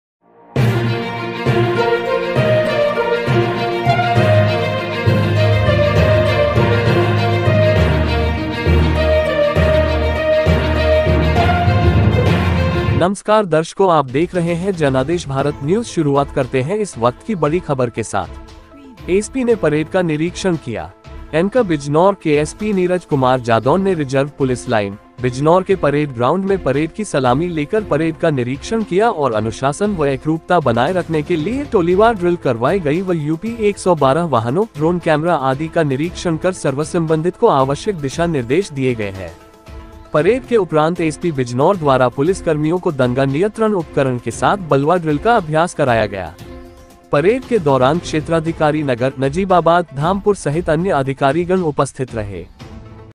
बिजनौर की बड़ी खबर||पुलिस अधीक्षक बिजनौर ने||पुलिस परेड का किया निरीक्षण#जनादेश
Document from Ramgopal